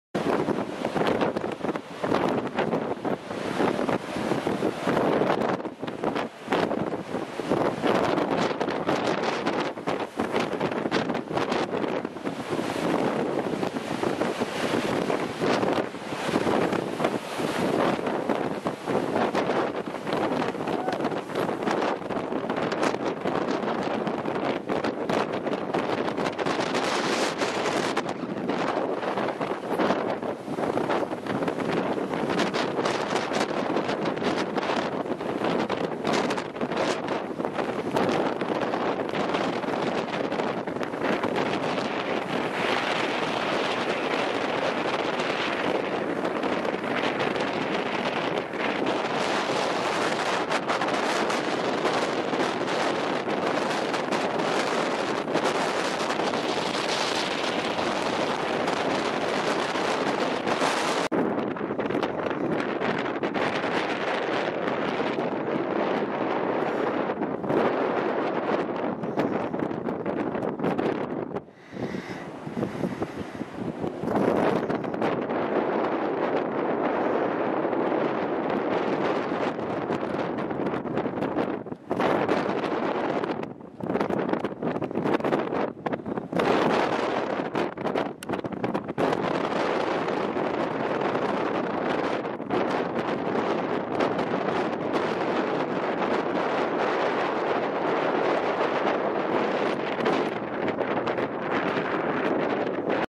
Hurricane Sandy - Atlantic City NJ - Before & After - Inlet Boardwalk 1
Before & After of the Worst Hurricane to Hit the East Coast in History. The before was taken Sept 28th, the after November 3rd, Less than 7 days later!